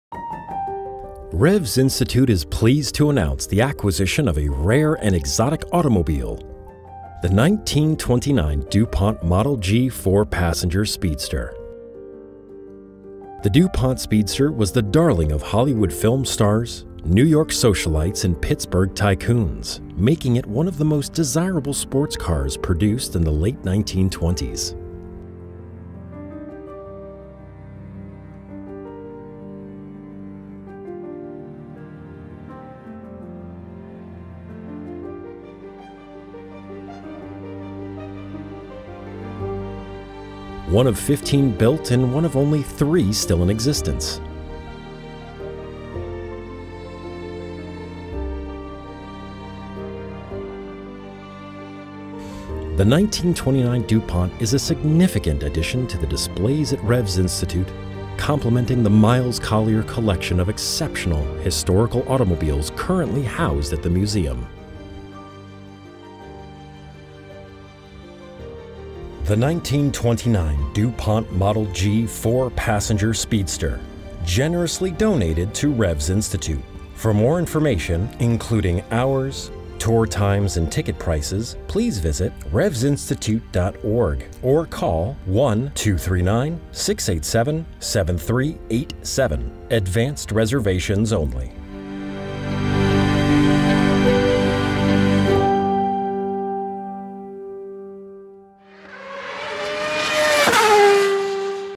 Revs Presents: 1929 Du Pont Model G four-passenger Speedster
Revs Institute is pleased to announce the acquisition of a rare and exotic automobile: a 1929 Du Pont Model G four-passenger Speedster. This is a historically significant car and very few examples remain in existence.

The Du Pont Speedster epitomized the romance of the movies in the 1920s and was the darling of Hollywood film stars, New York socialites and Pittsburgh tycoons. It was one of the most desirable sports cars produced in the late 1920s.

In 1919, E. Paul duPont began his American motorcar venture by assembling the finest outsourced components available into luxurious and sophisticated cars. When Mary Pickford, popular silent movie star, bought an early two-seater for her dashing husband and famous swashbuckler, Douglas Fairbanks, the new marque's prestige was assured.

Now, all that was lacking was racing credibility. Du Pont entered a four-seat Speedster in the 1929 Le Mans 24-hour race and, though running a solid 8th overall, it was forced to retire with handling and transmission woes. Du Pont also entered a two-seat model of the Speedster for the 1930 Indy 500 race. A spin and crash on the 22nd lap of the Indy 500 race put an end to Du Pont’s racing ambitions. But, as intended, competition had made the Model G sporting news.

The donated Speedster is one of 15 built and only one of three still in existence. The car won its class at the prestigious Pebble Beach Concours d’Elegance, in 2007, after having been beautifully restored.

Miles C. Collier, Founder of Revs Institute said: " This rare automobile is a significant donation to Revs Institute. We are most grateful to Mr. Thomas L. duPont for his generosity. This is an exciting addition to the displays at Revs Institute and will aid in our mission: to drive the recognition of the automobile as not only a great social change agent, but also one of the highest expressions of all that is great in the human mind and spirit."

As a not-for-profit 501(c)(3) educational organization, Revs Institute® welcomes donations of all kinds, to benefit its mission and collections. The 1929 Du Pont provides a significant addition to the displays at Revs Institute and compliments the Miles Collier Collections of exceptional, historical automobiles, currently housed at the museum.

Mr. Thomas L. duPont's very generous donation will be on view to the public beginning Thursday, April 7.